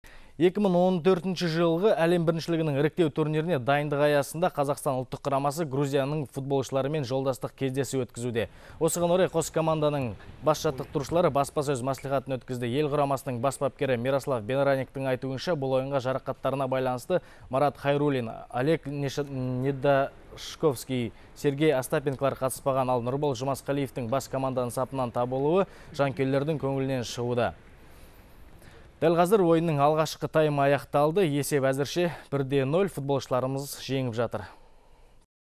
Футбол. Қазақстан - Грузия
2014 жылғы әлем біріншілігінің іріктеу турниріне дайындық аясында Қазақстан ұлттық құрамасы Грузияның футболшыларымен жолдастық кездесу өткізуде.